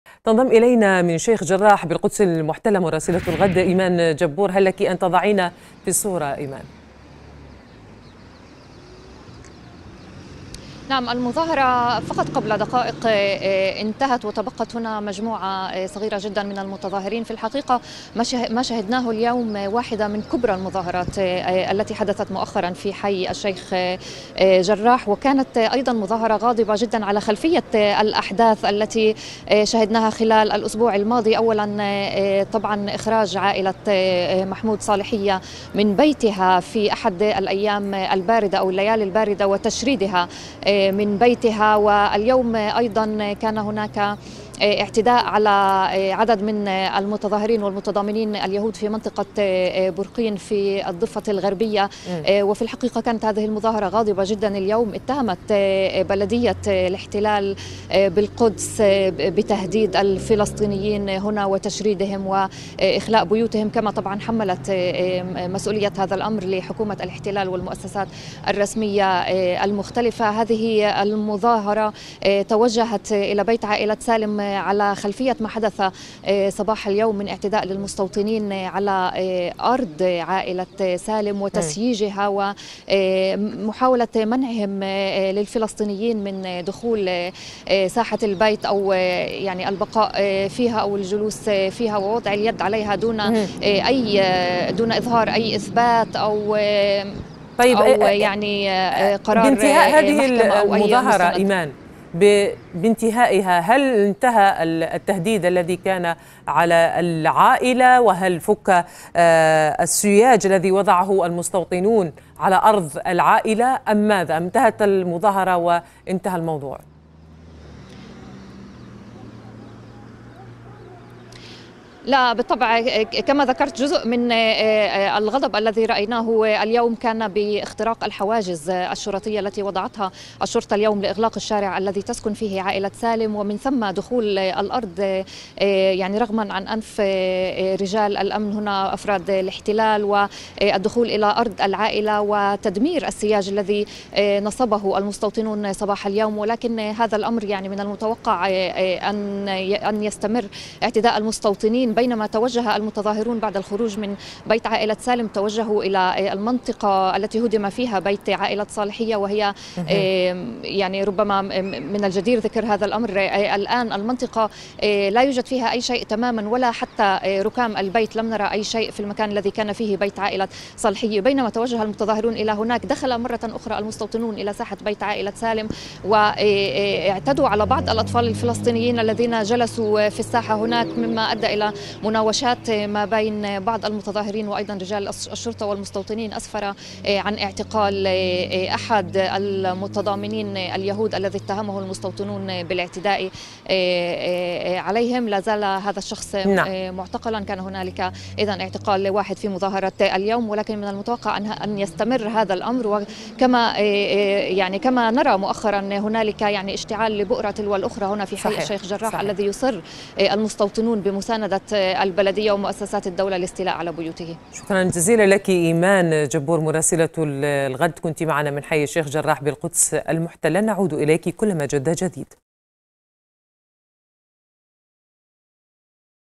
مظاهرة غاضبة في الشيخ جراح تضامنا مع عائلة سالم المهددة بالإخلاء
تضامنا مع عائلة_سالم المهددة بالإخلاء، نظم ناشطون فلسطينيون ومتضامنون إسرائيليون مظاهرة حاشدة انطلقت من حي_الشيخ_جراح في القدس المحتلة وتوجهت إلى بيت عائلة سالم.
وذلك رغم الإغلاقات التي فرضتها الشرطة منذ الصباح على الحي عبر نصب الحواجز الحديدية. وكانت عائلة سالم قد تعرضت صباح اليوم الى اعتداء من المستوطنين الذين سيّجوا أرض العائلة تمهيدا للاستيلاء عليها.
المزيد من التفاصيل مع مراسلة الغد من الشيخ جراح بالقدس المحتلة إيمان جبور.